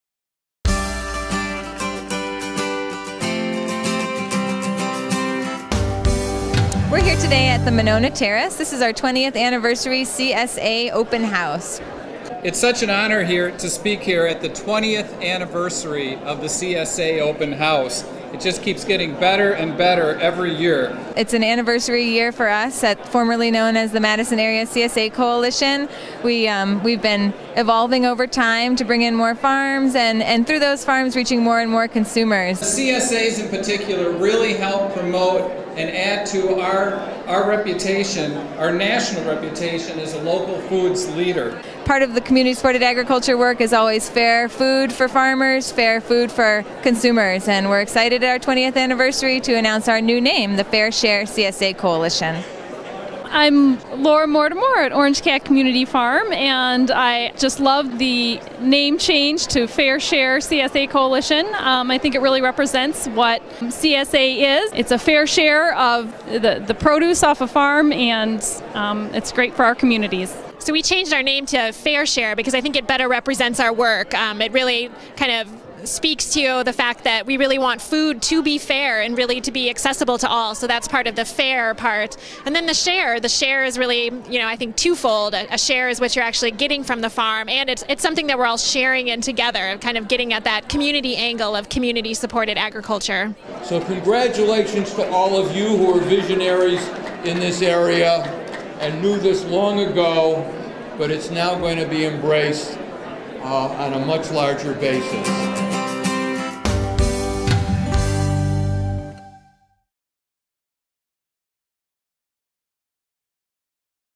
MACSAC changes name to Fairshare CSA
MACSAC announced their name change to Fairshare CSA. The change represented how the organization has grown over the last 20 years to become a state wide supporter of community sharing in agriculture. Speakers include Kiera Mulvey Fairshare, Joe Parisi Dane County Executive, and Madison Mayor Paul Soglin.